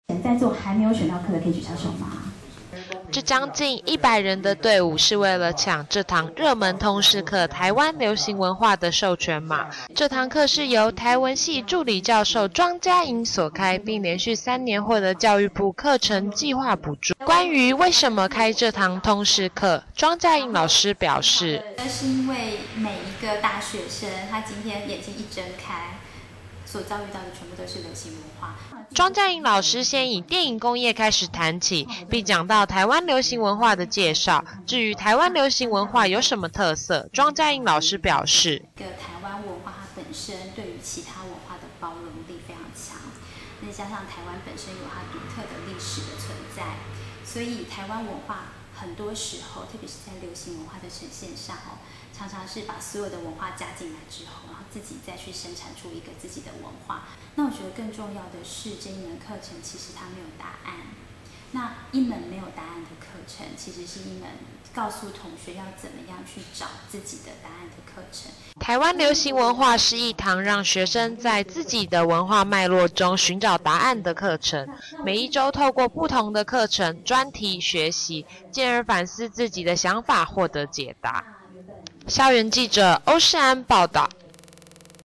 1010913熱門通識專題》臺灣流行文化 認識我們生活的地方.wmv
搶著要索取授權碼的隊伍，從講台排到教室尾端，再轉一個彎到門外。這堂吸引近百位學生爭相修習的熱門通識課程「台灣流行文化」，由台灣語文學系助理教授莊佳穎老師開設，在星期四的七、八節於教育大樓202演講室上課。該課程更榮獲教育部「100學年度第1學期公民核心能力課程改進計畫---績優夥伴課群發展計畫」補助。